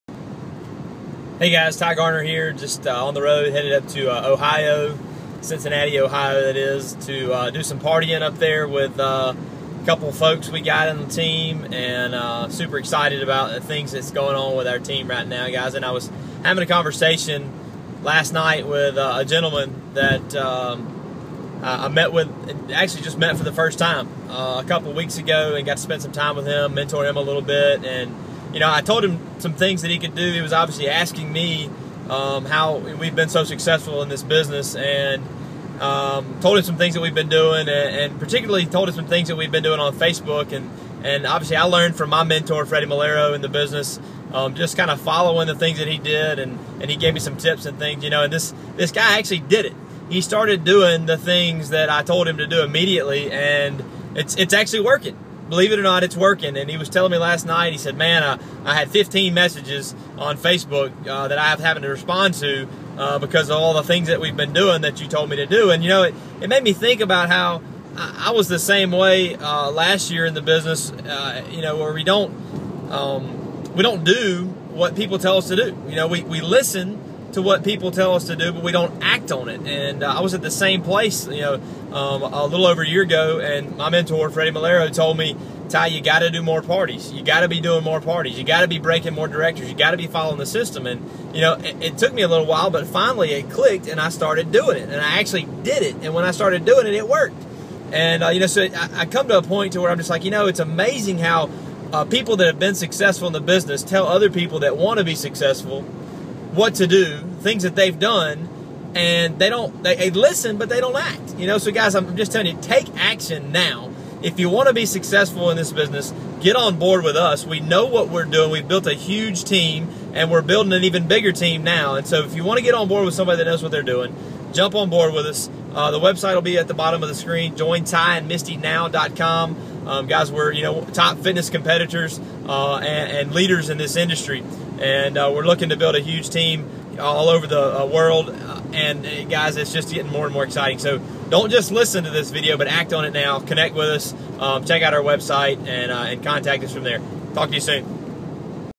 Don't just listen, Act!!
Hear 1 Star Ambassador & Top Visalus Leader Ti Garner explain the importance of ACTING on what you hear from people that have what you want & why you should join our team now as we spread The Challenge message across the country Get a free sample